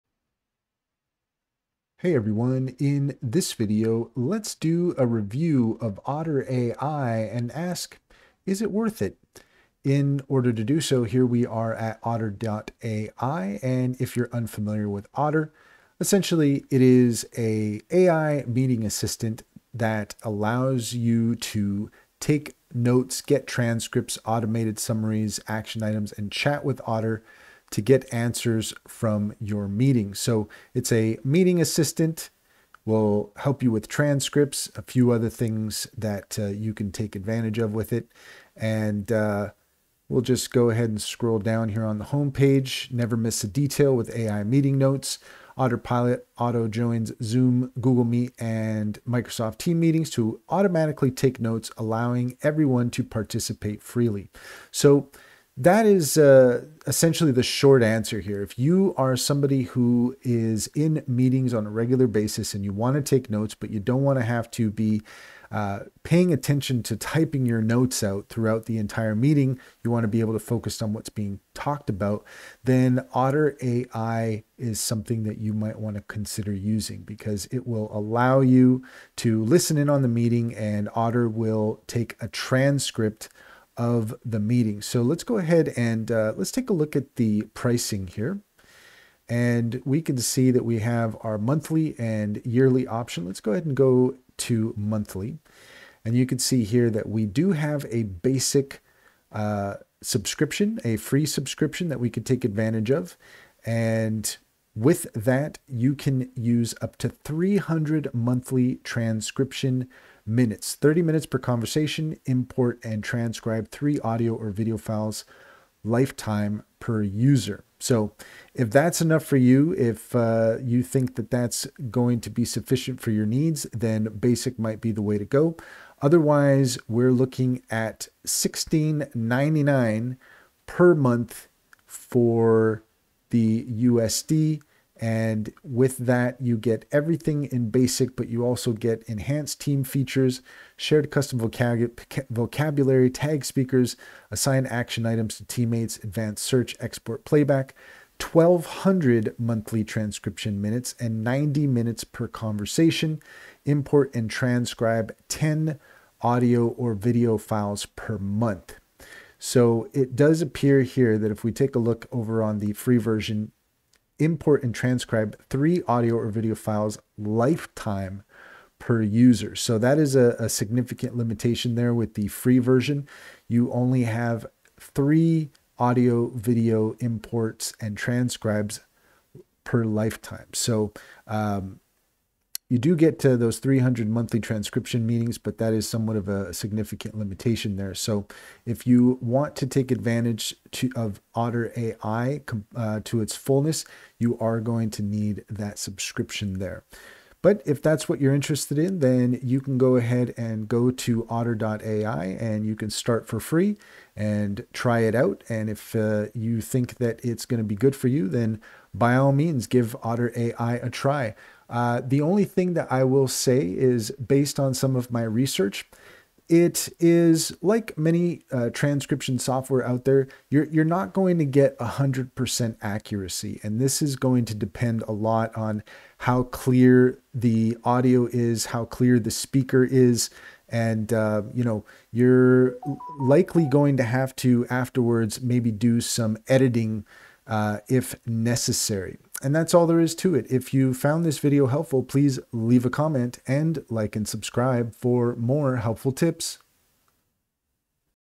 Otter Ai Review - Is It Worth It? (2025)
Get a detailed Otter AI review covering its transcription accuracy, features, pricing, integrations, and overall performance to help you decide if it’s the right tool for you.